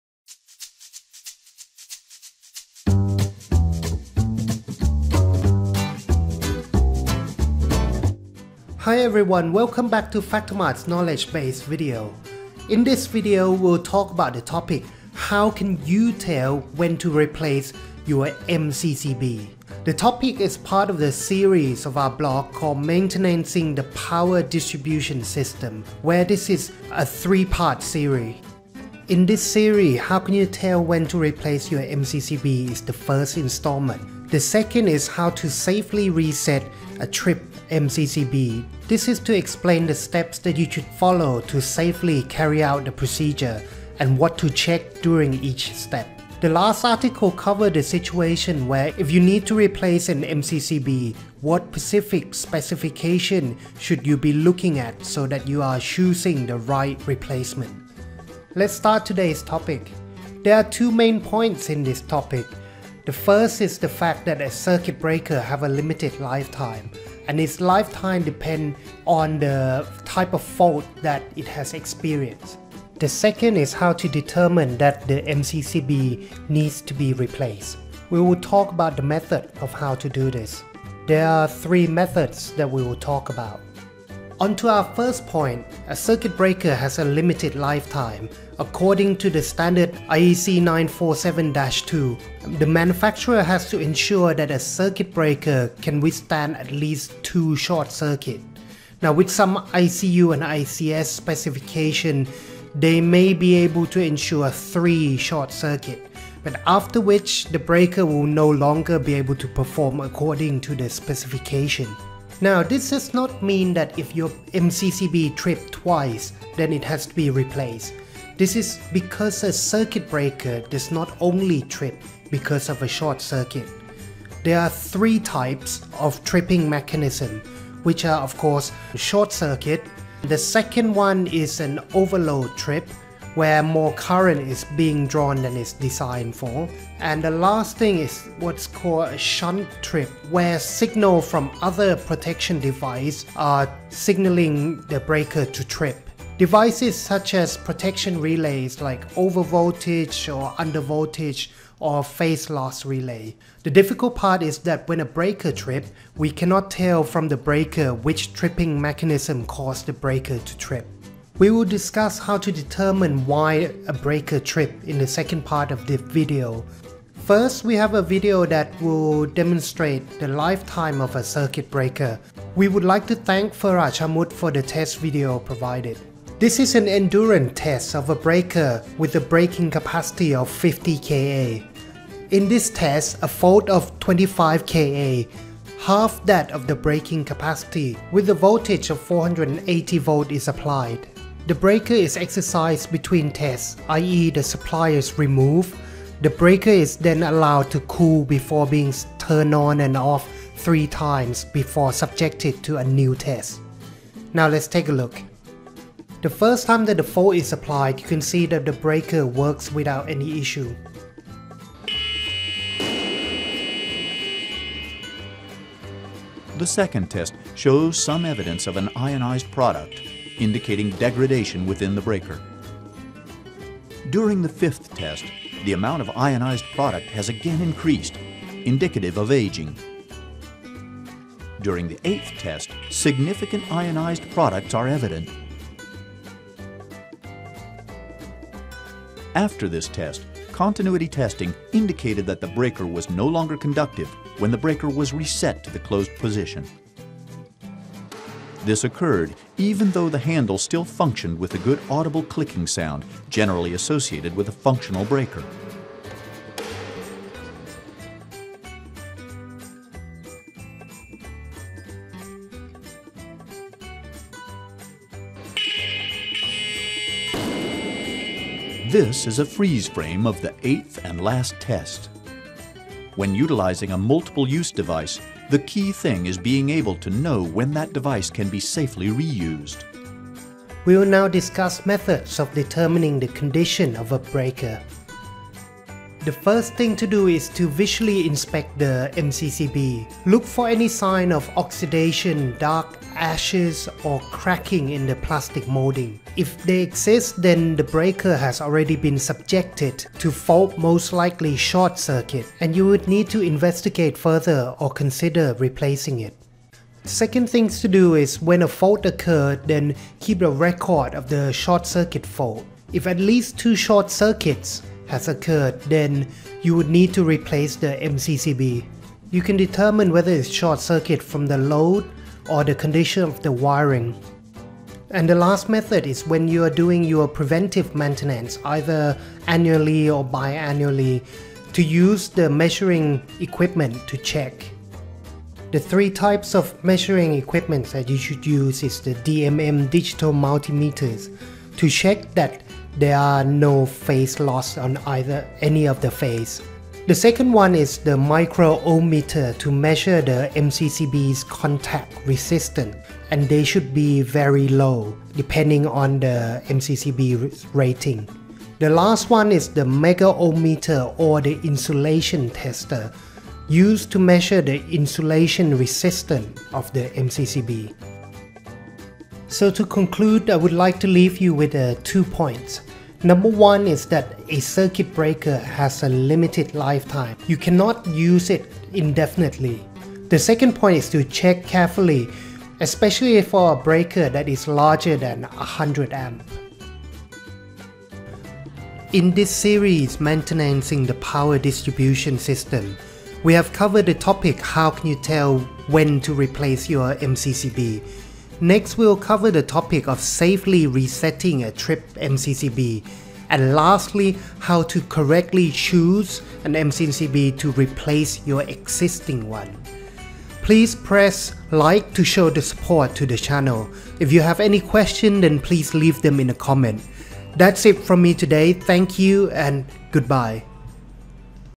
How can we tell if the MCCB needed changing
The Molded Case Circuit Breakers (MCCB) have a limited lifetime when subjected to the short circuit faults. So how can you tell that it is time to change your MCCB to ensure proper protection of your electrical system? In this article, we will discuss 3 methods of inspection to determine the right time to change your MCCB.